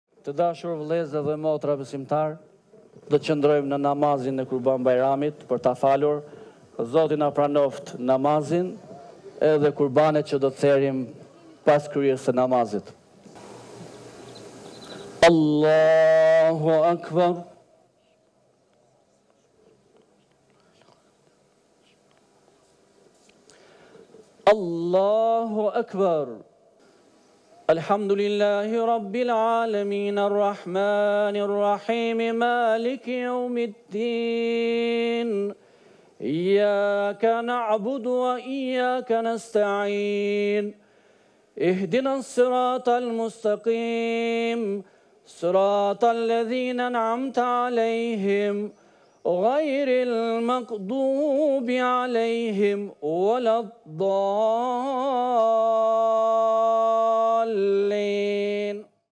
"Kurban Bajrami" - Falja e Namazit në sheshin "Skënderbej" ( 28 Qershor 2023 )
Besimtarët myslimanë kremtojnë sot festën e Kurban Bajramit. Falja e namazit të Kurban Bajramit në Tiranë u krye në sheshin Skënderbej.

Kryetari i Komunitetit Mysliman Shqiptar Bujar Spahiu, iu drejtua mbarë shqiptarëve duke theksuar se Kurban Bajrami është festë e sakrificës.

Në këtë ditë të gjithë besimtarët myslimanë përcjellim mesazhe paqeje e harmonie për njëri-tjetrin si dhe lutje për më shumë begati. ORA News iu uron gëzuar të gjithë besimtarëve myslimanë. Aktiviteti ka nisur në orën 05:30 të mëngjesit dhe do të zgjasë deri në orën 07:30.

Pas përfundimit të faljes së Kurban Bajramit do të bëhet edhe lirimi i hapësirës publike dhe sheshi “Skënderbej” do i rikthehet aktivitetit normal për t’u shfrytëzuar nga qytetarët.